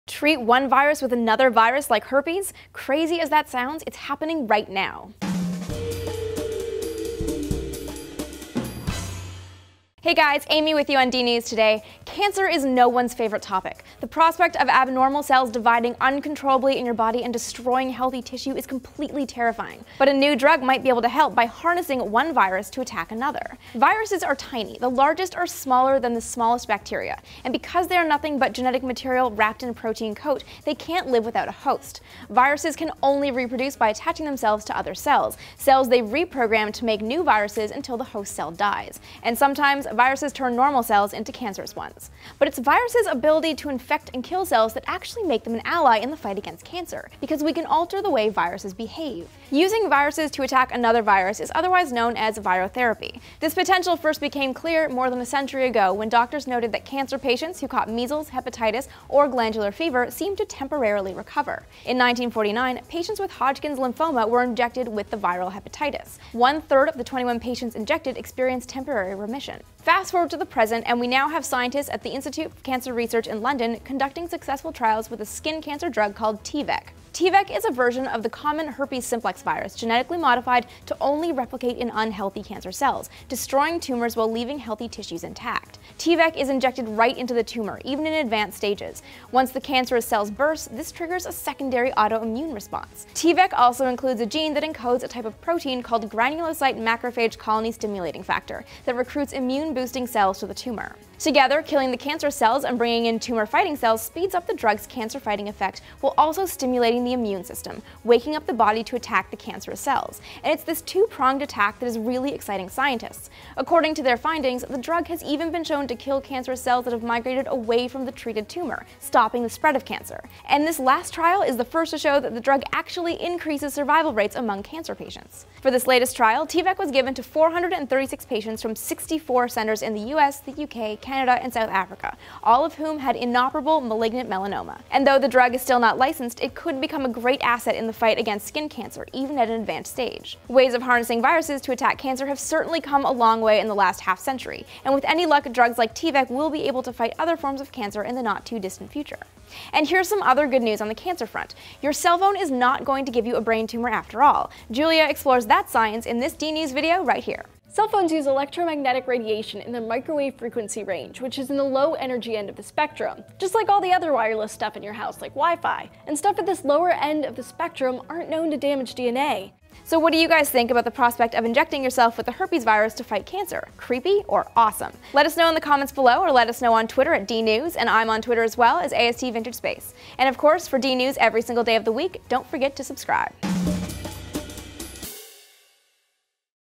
Can The Herpes Virus Kill Cancer?
Doctors recently modified the herpes simplex virus to fight skin cancer. Could the ability to kill cells by using viruses be the key to curing cancer for good?

Virotherapy: skin cancer successfully treated with herpes-based drug
“In the trial, involving more than 400 patients with aggressive melanoma, one in four patients responded to the treatment, and 16% were still in remission after six months.”

“The modified herpes virus is harmless to normal cells but when injected into tumours it replicates and releases substances to help fight the cancer.”

“ T-VEC is made from a modified herpes bug and would likely be the first among a number of virus-based cancer treatments in the pharma pipeline to be approved.”